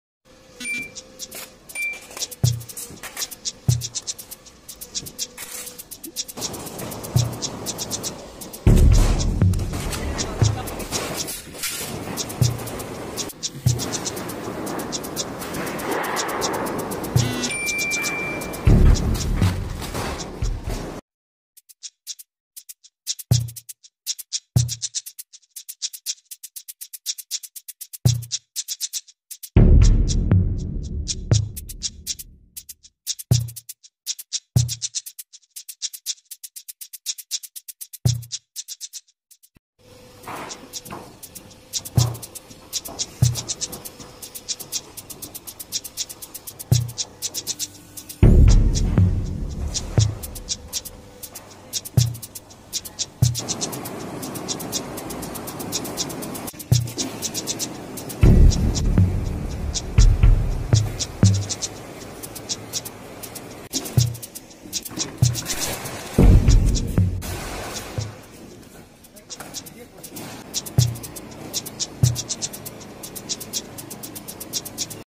JSW COLOURON PLUS roofing sheet process
From packed coil to a full finished profile sheet, here we have JSW COLOURON+ brand.
Specs - 0.50 AZ 150 BARE (JSW VISHWAS )